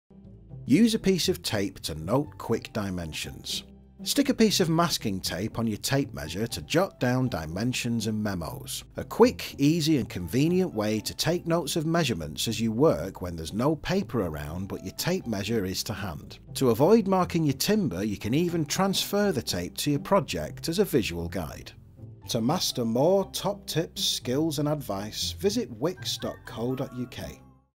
Wickes Top Tips - Use a piece of tape to note quick dimensions
Stick a piece of masking tape on your tape measure to jot down dimensions and memos. A quick, easy and convenient way to take notes of measurements as you work when there’s no paper around, but your tape measure is to hand. To avoid marking your timber you can even transfer the tape to your project as a visual guide.

If you'd like to find out more from Wickes, the order line number is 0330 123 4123